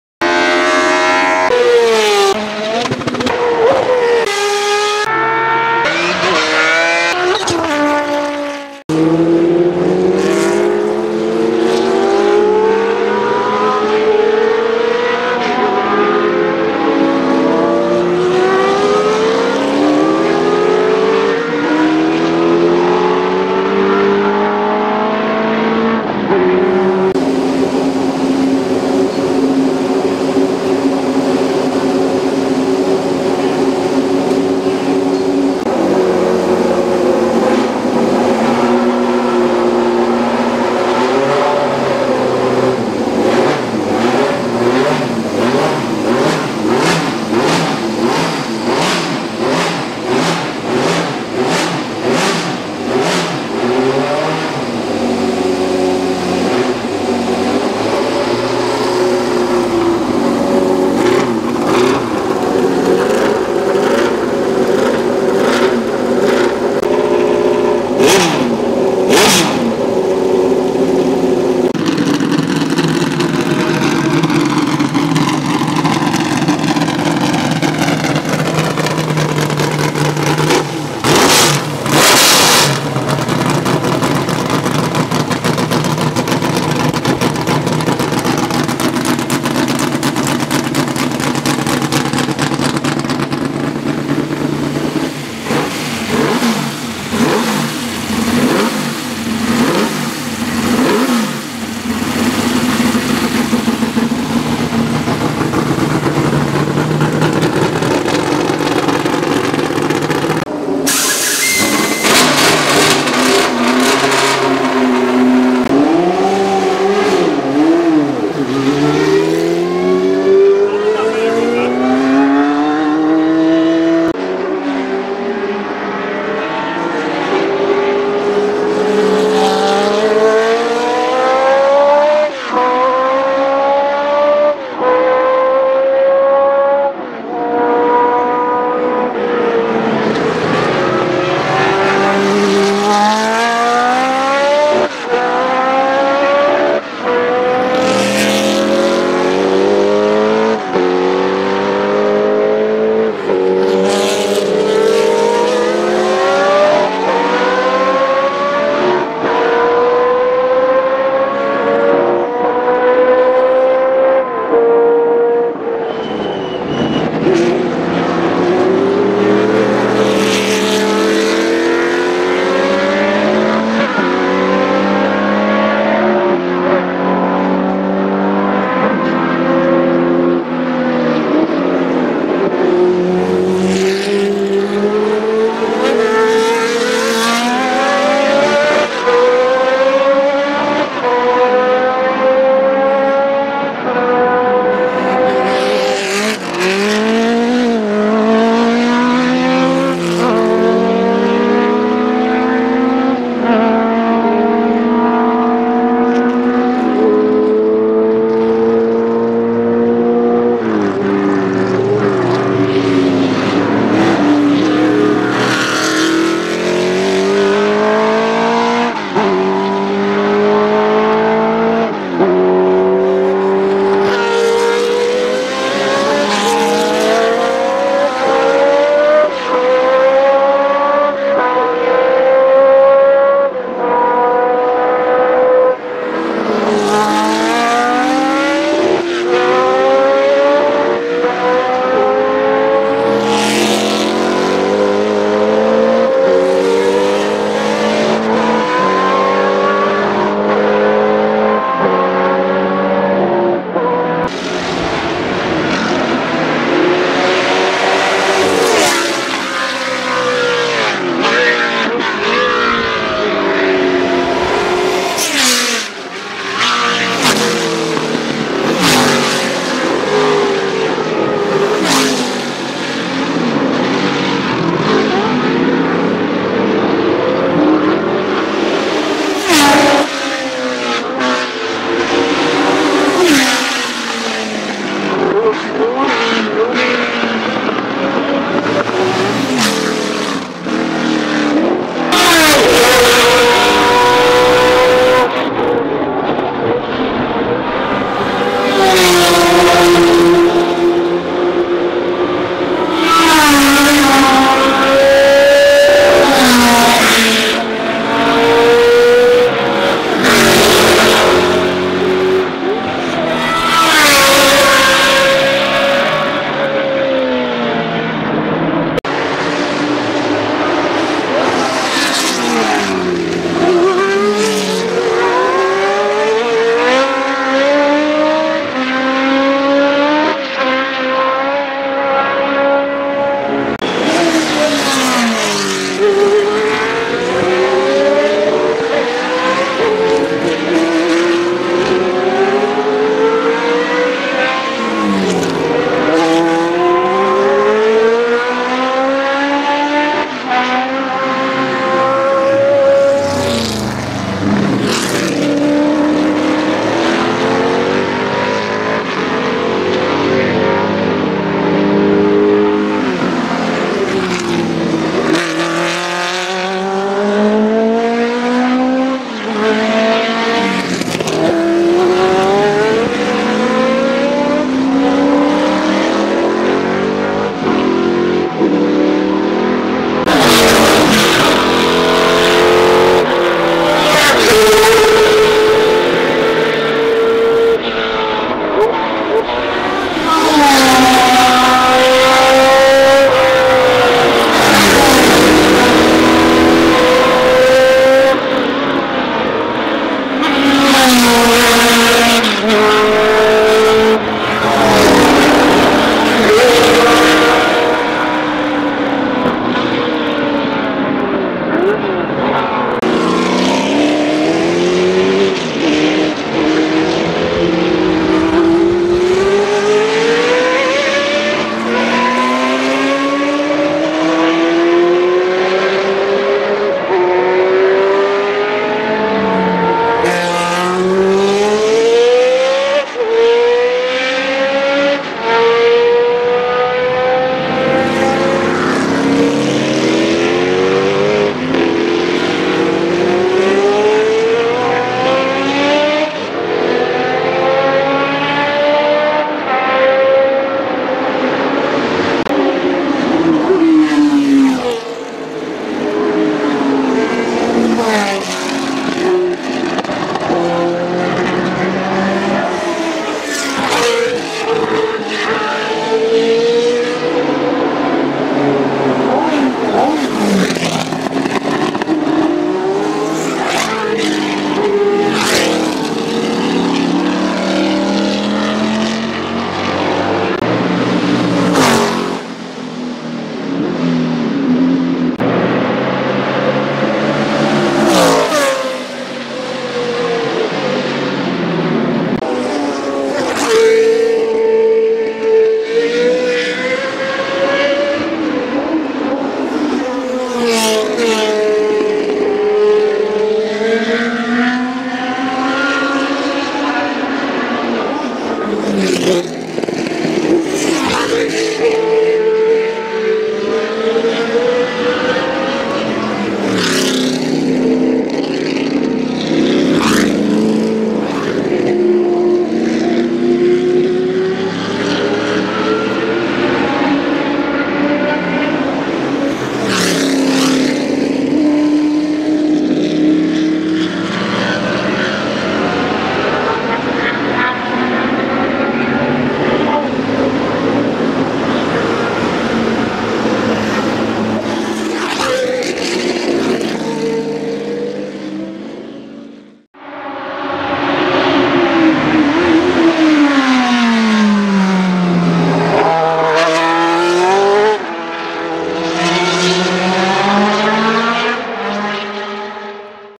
Classic F1 and sportscars testing at Circuit Zolder
This weekend the Historic GP will be held at Zolder, so many teams were already testing their cars yesterday.
Lots of 70s / 80s F1s, sportscars and some touringcars were on track.
Check this site for more info.

Track: Circuit Zolder
Event: Testday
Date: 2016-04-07

Filmed with
Canon Legria HF M46 + DM-100 microphone
Canon EOS 7D + Røde Videomic Pro